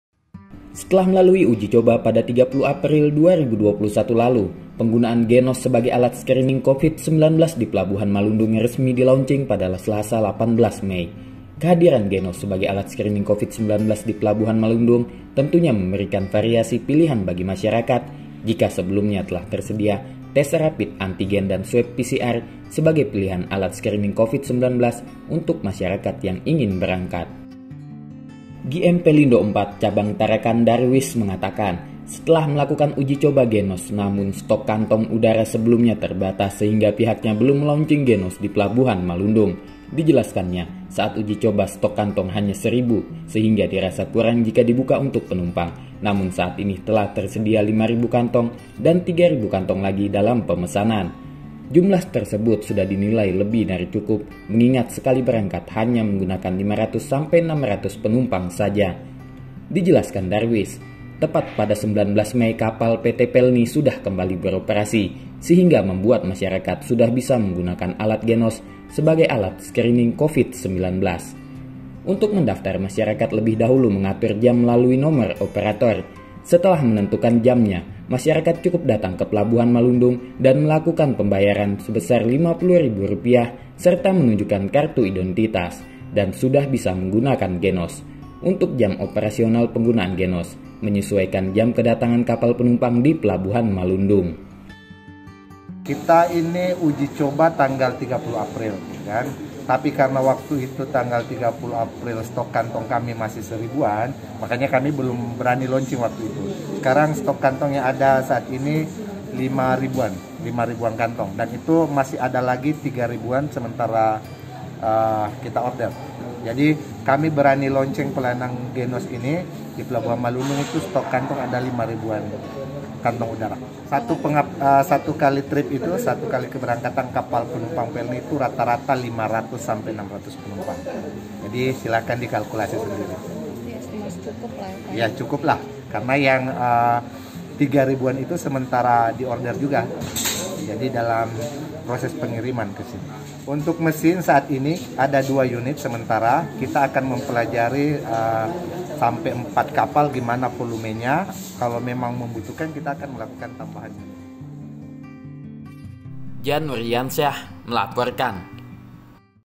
Transportasi Laut Pakai GeNose C19
Setelah melalui uji coba pada 30 April 2021 lalu, penggunaan GeNose sebagai alat screening Covid-19 di Pelabuhan Malundung resmi di-launching pada Selasa 18 Mei. Kehadiran GeNose sebagai alat screening Covid-19 di Pelabuhan Malundung tentunya memberikan variasi pilihan bagi masyarakat, jika sebelumnya telah tersedia tes rapid antigen dan swab PCR sebagai pilihan alat screening Covid-19 untuk masyarakat yang ingin berangkat.